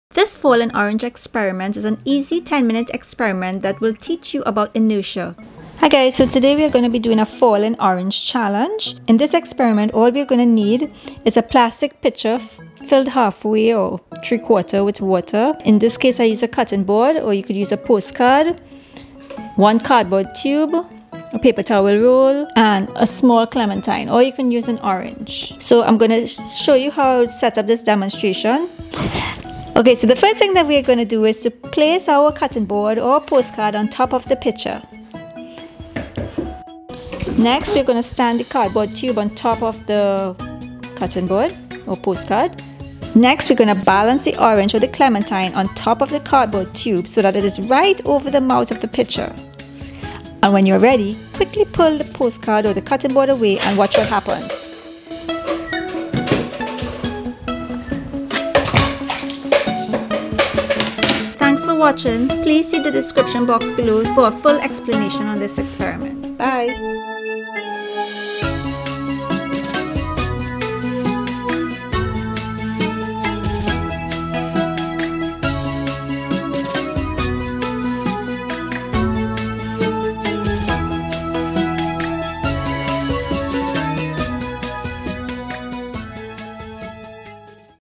INERTIA - Falling Orange Experiment
Have you ever seen the trick where someone pulls a tablecloth out from under a bunch of dishes without breaking them? Have you ever wondered how this is possible?
This experiment is a similar and less expensive trick that will surely impress your friends and teach them about inertia at the same time.

MATERIALS:
1 pitcher of water
1 plastic cutting board OR post card OR stiff cardboard
1 cardboard tube OR paper towel roll
1 orange OR clementine

Inertia is a property of matter that causes it to resist changes in velocity (speed and/or direction). According to Newton's first law of motion, an object with a given velocity maintains that velocity unless acted on by an external force.

In this case, the inertia of the orange / clementine is so great because of its weight that it is not carried along with the fast movement. The light cardboard tube, on the other hand, flies off because its inertia is low.